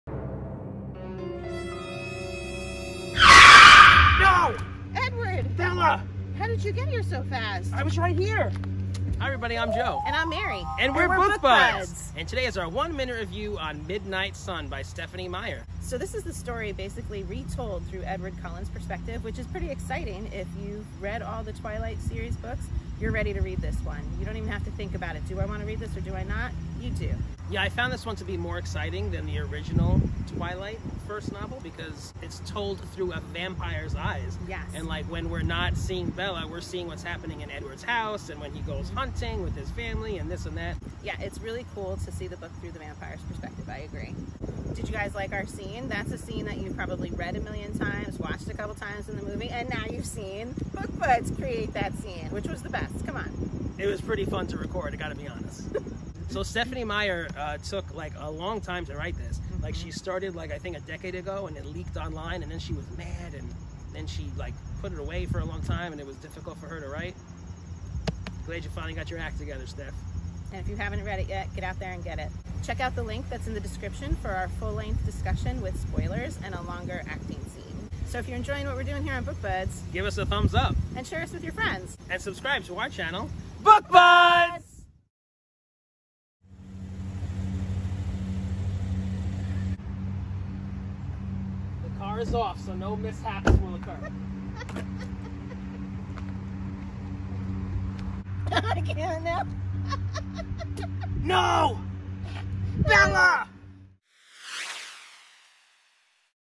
One Minute Book Review: Midnight Sun by Stephenie Meyer - NO Spoilers
NO Spoilers!  Book Buds do a One Minute Review of the book Midnight Sun by Stephenie Meyer!  For a longer discussion with spoilers and extended, hilarious reenactment scenes for this book, check out this Did you read this book? Let's chat in the comments!